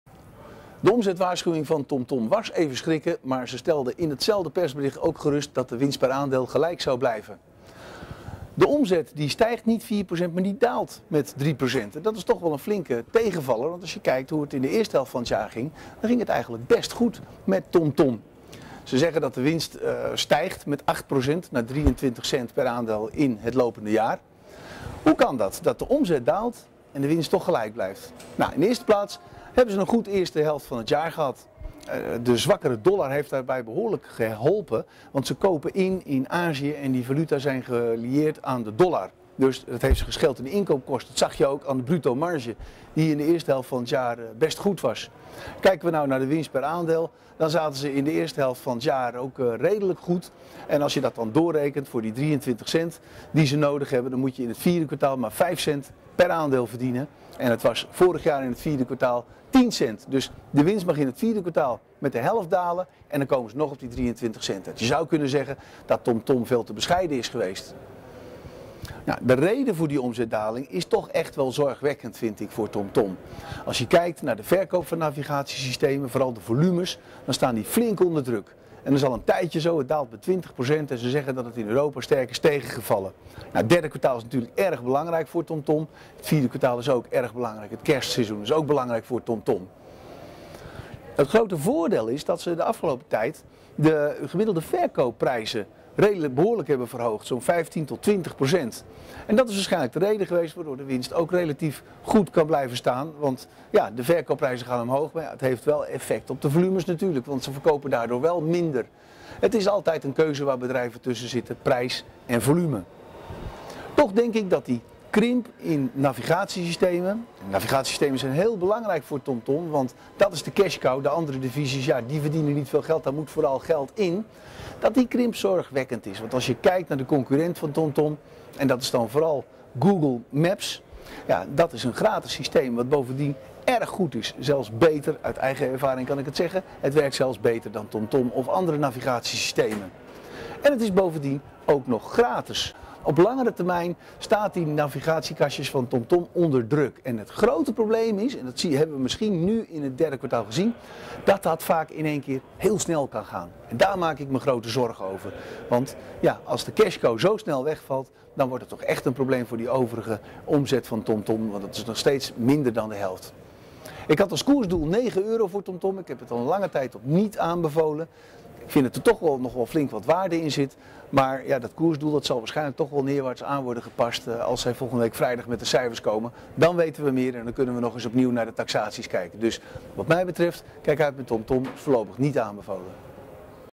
Gehandhaafde winstverwachting TomTom zeker haalbaar
Krimp in verkoop navigatiekastjes wel zorgwekkend volgens Jos Versteeg van Theodoor Gilissen.